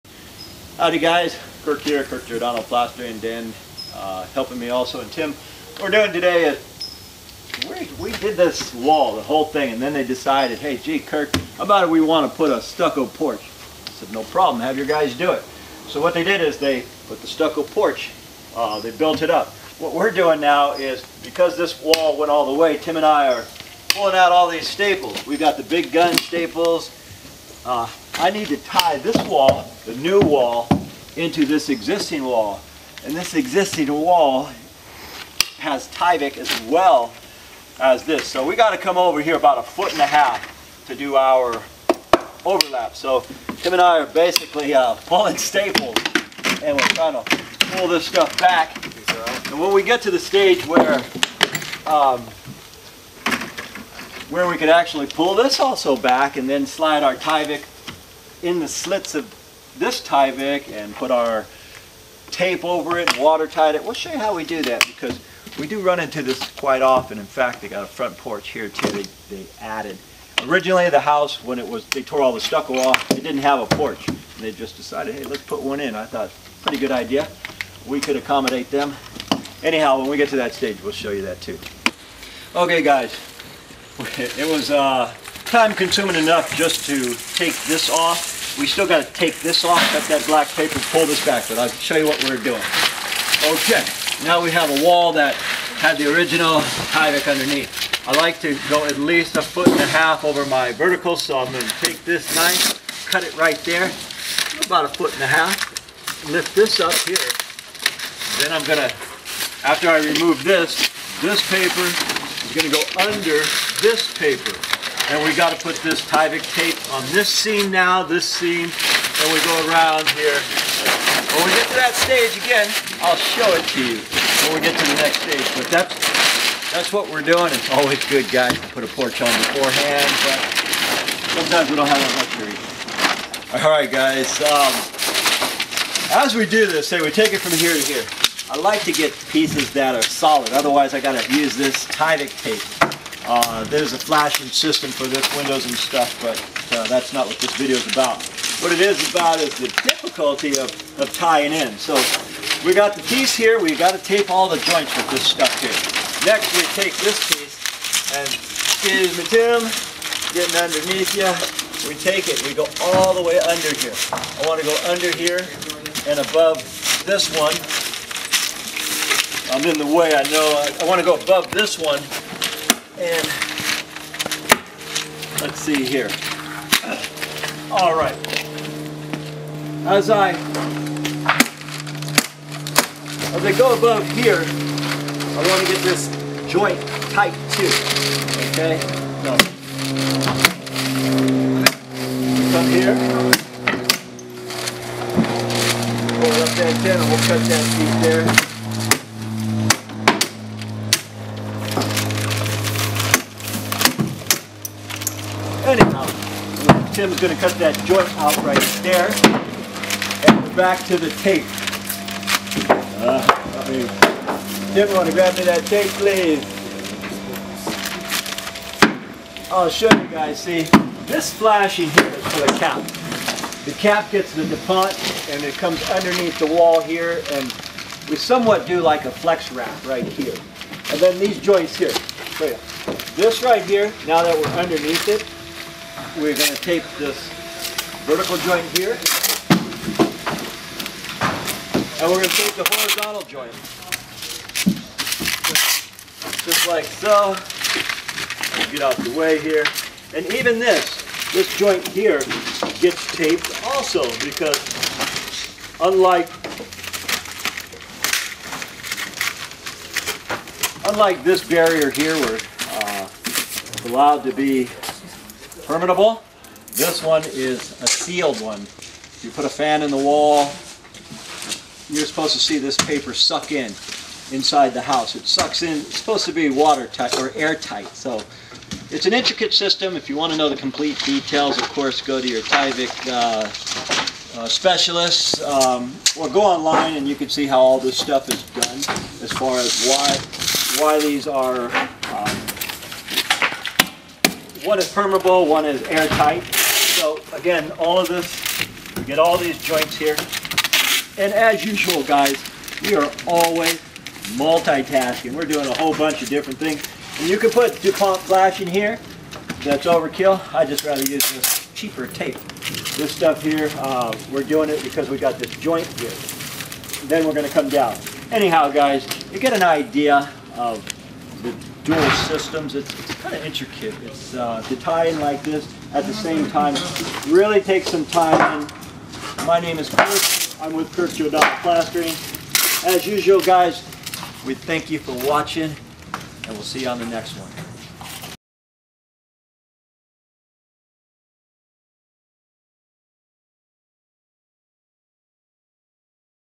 Tyvek Stucco Wrap with two layers of 60 minute paper
In this video, we're Installing  Stucco Wrap over plywood sheathing.
Then attaching two layers of 60-minute paper over it..

Is it possible to integrate Tyvek Stucco Wrap with two-ply building paper? Yes, seeing is believing.
I
 These moister barriers can be purchased at Lowes or Home Depot, or call “Plastering companies” in your area to buy them. Such as Westside building supplies.

FYI, all a person needs for the proper moisture barrier are two layers of building paper when waterproofing homes in California.
As the homeowner requested, we added Tyvek stucco warp in this video. It was not really necessary.

 However, the fella who owns this house is an engineer and requested it.
 Have I done it before? About ten times over the years, usually on homes up in the hills for the wind chill factor.
In particular, the Skyline or Grizzly Peak area is located in Oakland, CA. But these extreme measures are used more in areas where there is much freezing rain, like Canada and the East Coast.